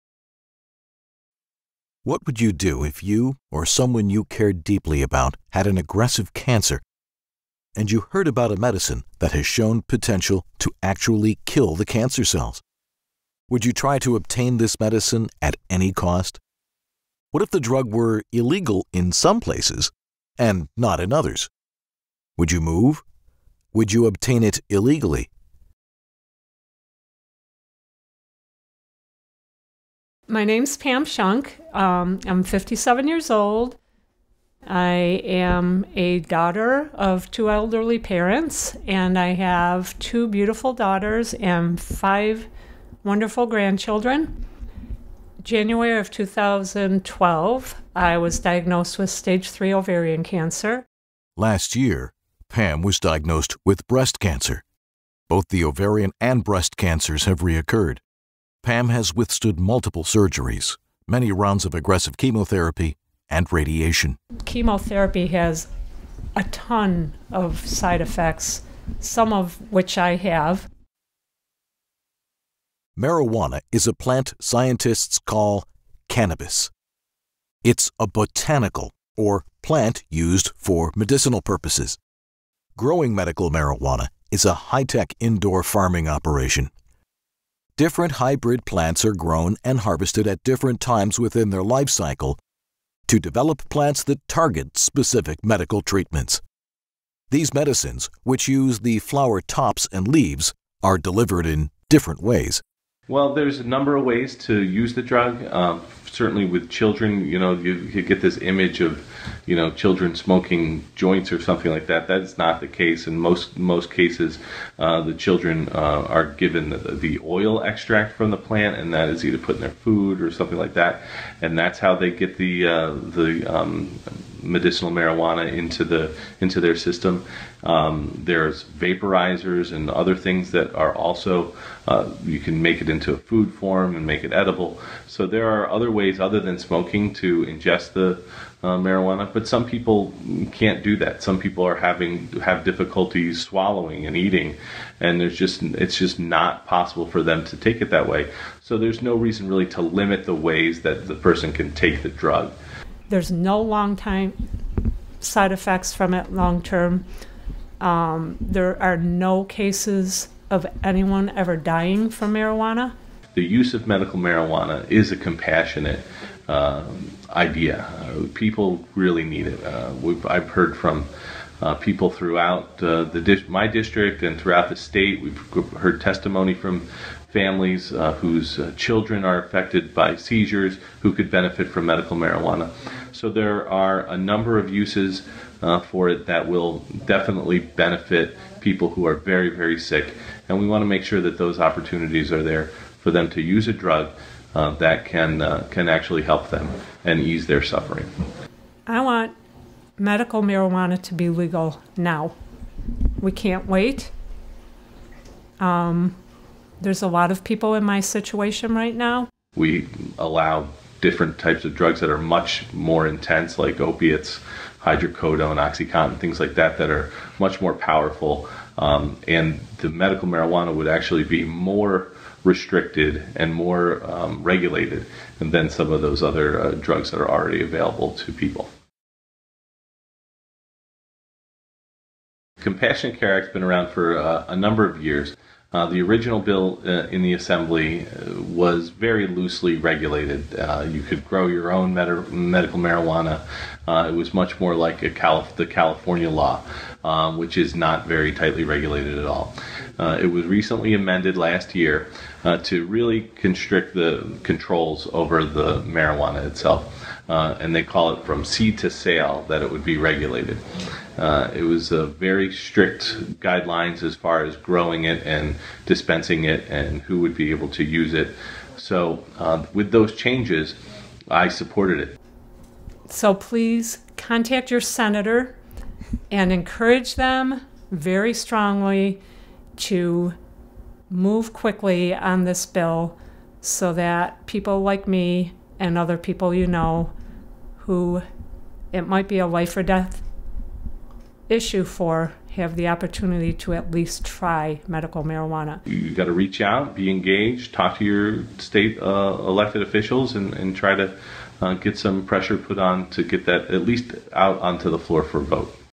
Pass The Compassionate Care Act In NYS!
An explanation by NYS Assemblyman Ray Walter of why The Compassionate Care Act should be passed by the NYS Senate!